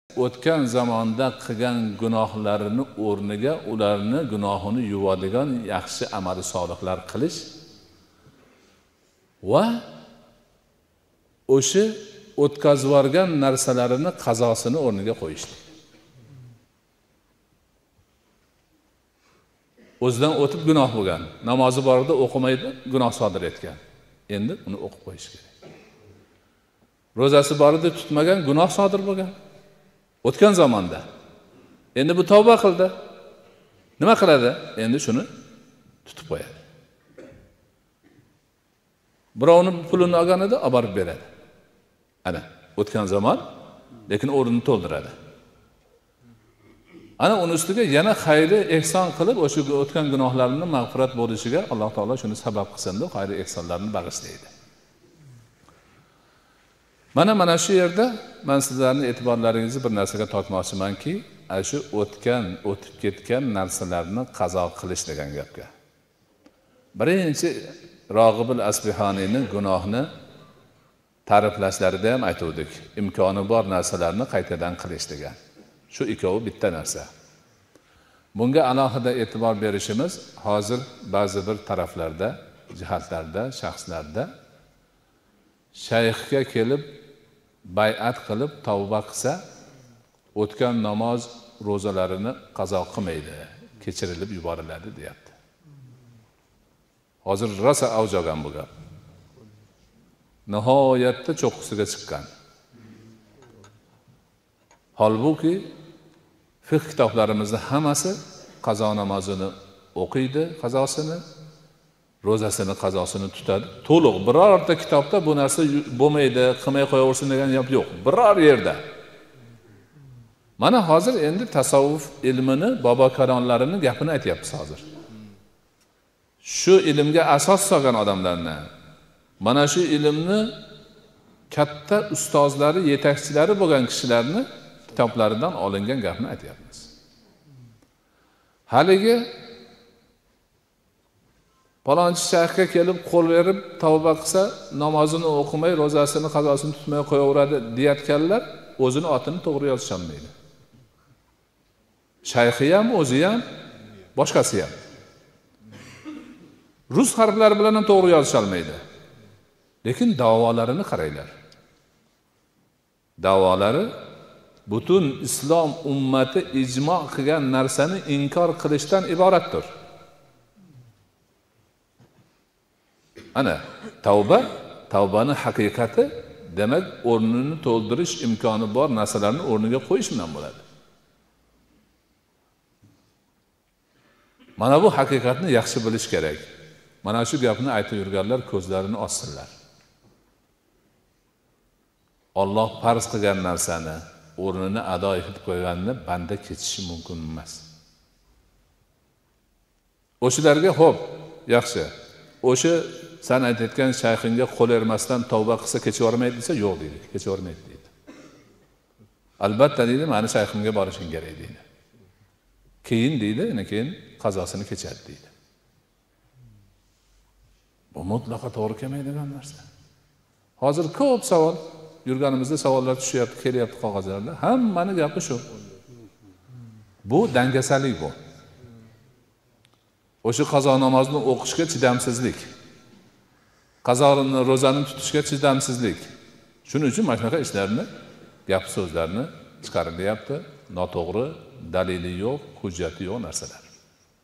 Oʻtgan gunohlarning qazosi va kafforoti | Ramazon suhbatlari
Shayx Muhammad Sodiq Muhammad Yusuf rahimahulloh

Ramazon suhbatlari turkumidan